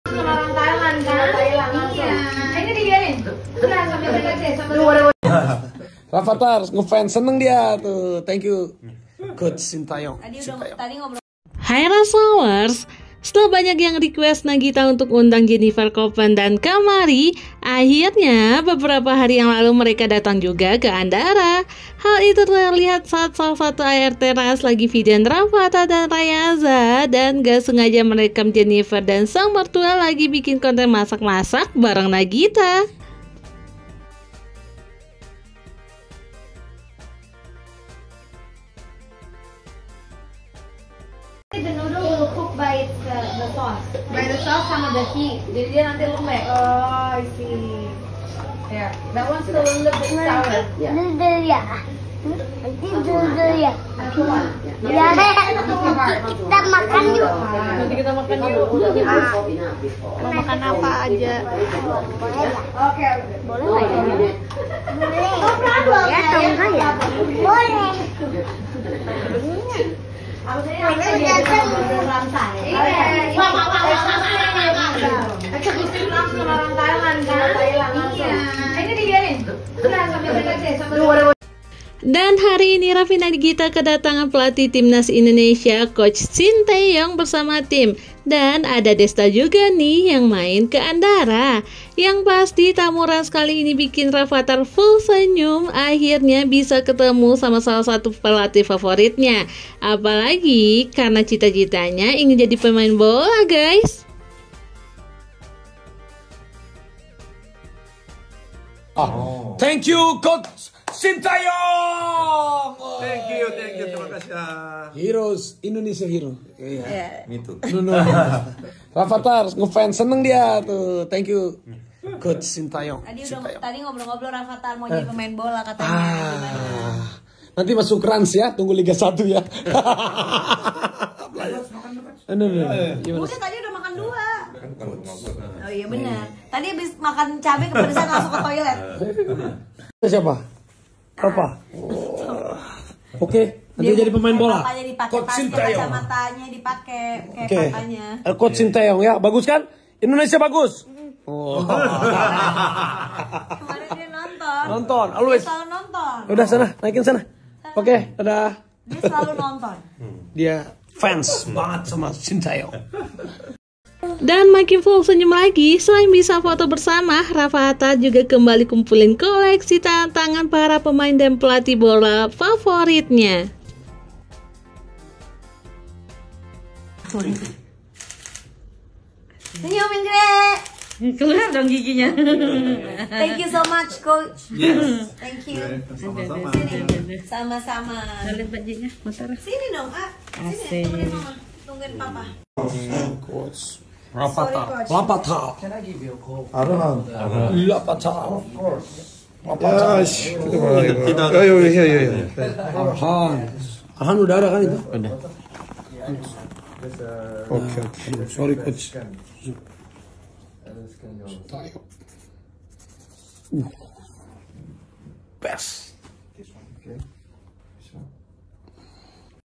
AKHIRNYA RAFFI NAGITA UNDANG JENNIFER COPPEN & KAMARI! RAFATHAR GIRANG DISMPERIN COACH SHIN TAE YONG
Video terbaru Rans edisi Raffi Nagita undang Jennifer Coppen Kamari dan Yaya ke Andara, akhirnya Raffi datangkan Coach Shintaeyoung